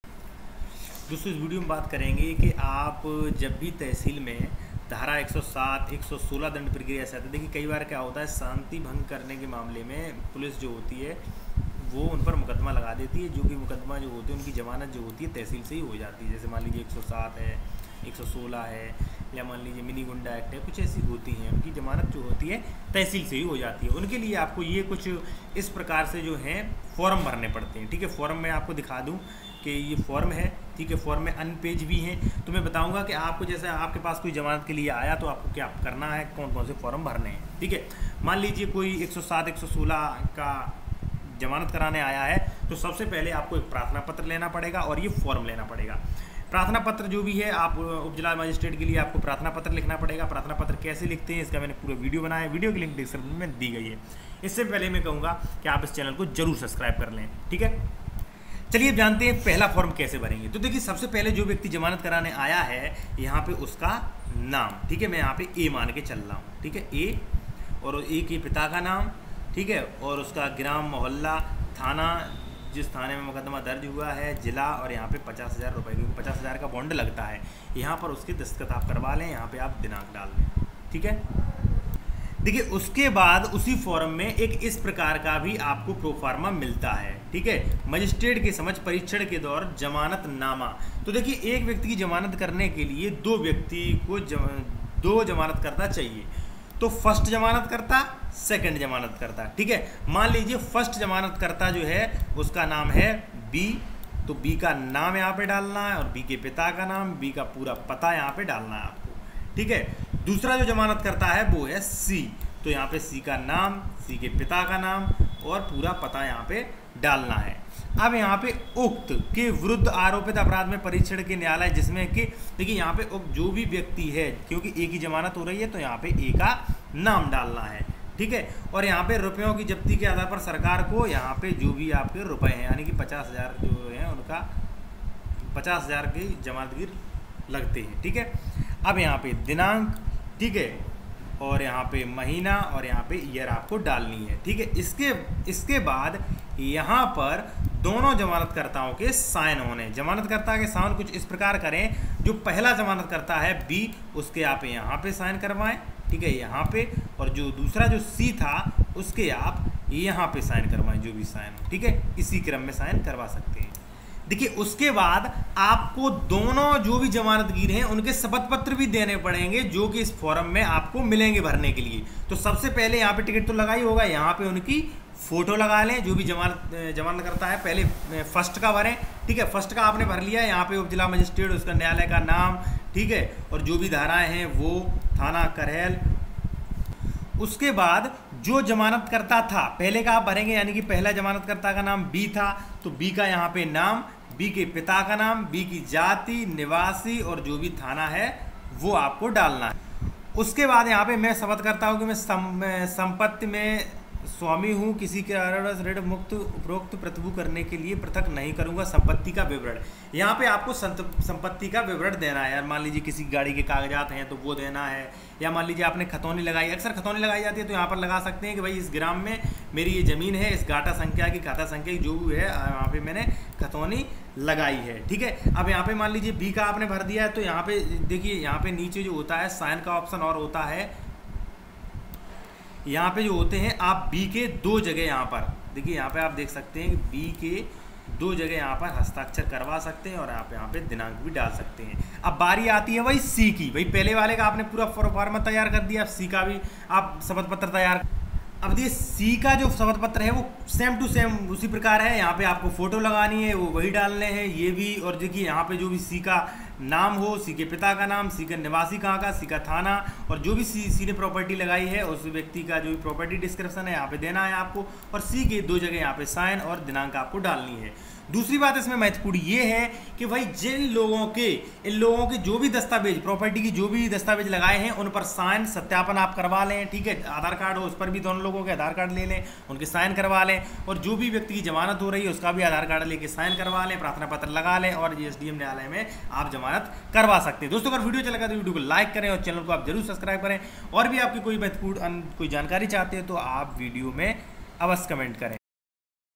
जमानत करवाने के लिए जमानत फॉर्म कैसे भरें जमानत फॉर्म 107,116 मुचलका कैसे भरें,
जमानत मुचलका फॉर्म भरने के लिए प्रारूप